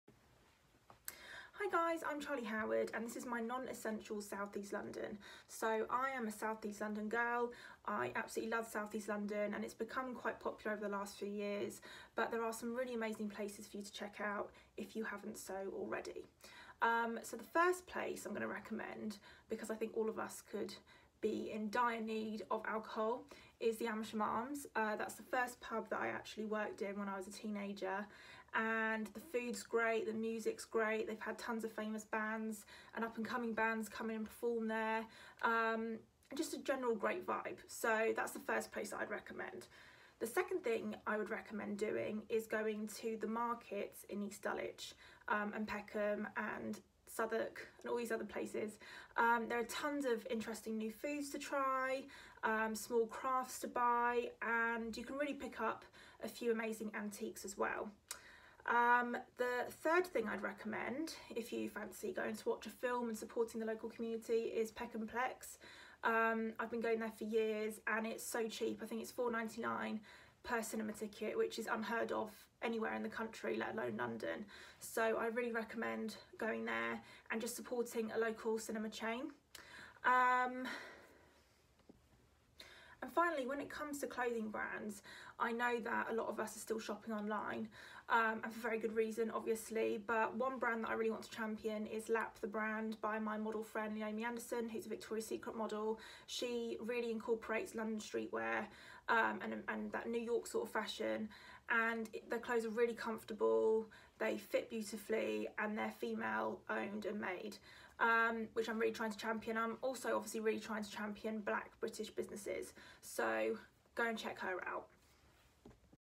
My Non-Essential: Charli Howard
With Non- Essential stores beginning to reopen from today, Charli Howard, Darren Kennedy, Ella Eyre, Grace Carter, Greta Bellamacina, Jack Guinness, Josephine De La Baume, Paloma Faith, Ramla Ali, Sabrina Elba, Jasmine Hemsley and Sydney Lima share with us the places they have missed and are looking forward to visiting again.